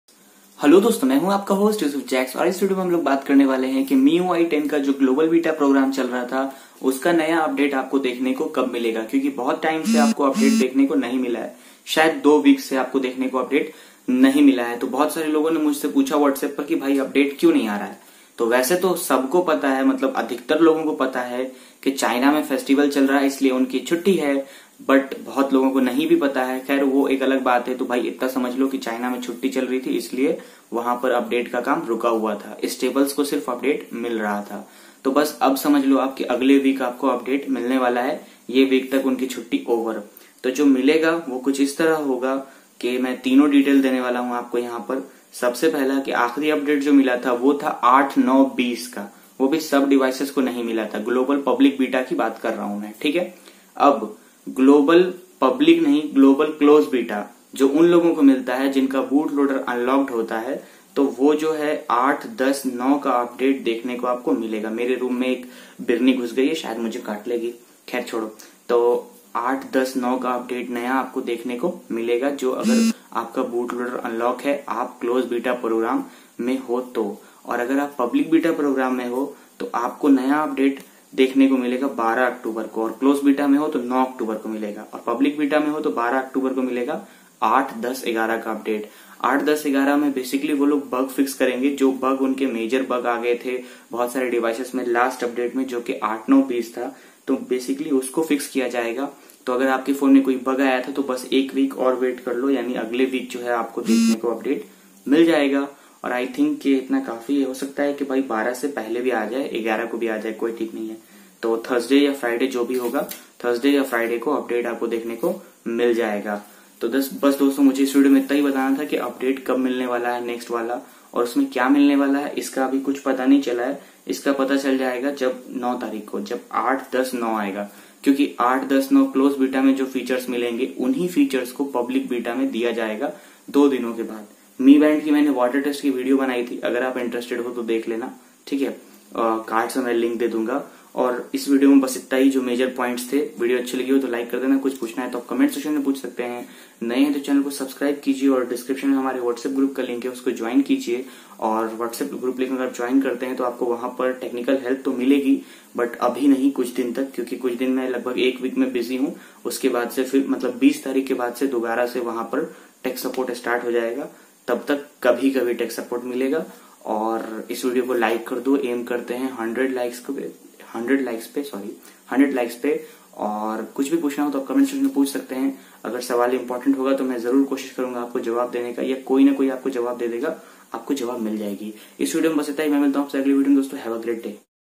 Why MIUI Stop Beta Program, MIUI 10 8.10.11 Beta Update Relese Date
AL WORKING MIUI 9 MIUI 10 ROM

use this link to get 100 in phone pe